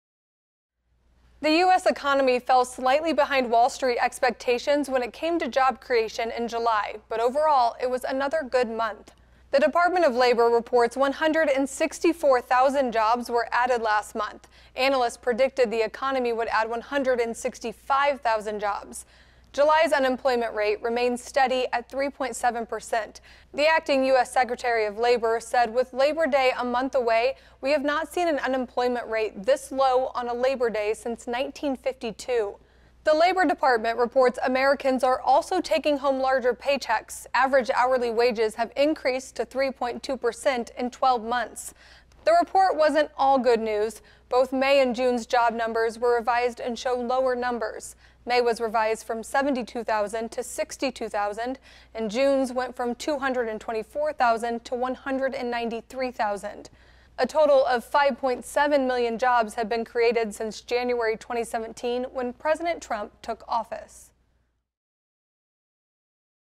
Unemployment rate remains steady in July
July's unemployment rate remained steady at 3.7%.